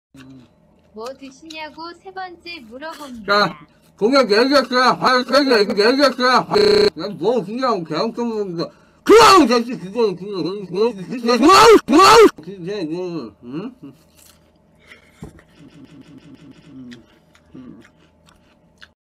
(케인 딥웹) 코울배
20220110 배먹방
이날 캐조씨는 케청자가 준 배를 맛있게 먹었다맨이야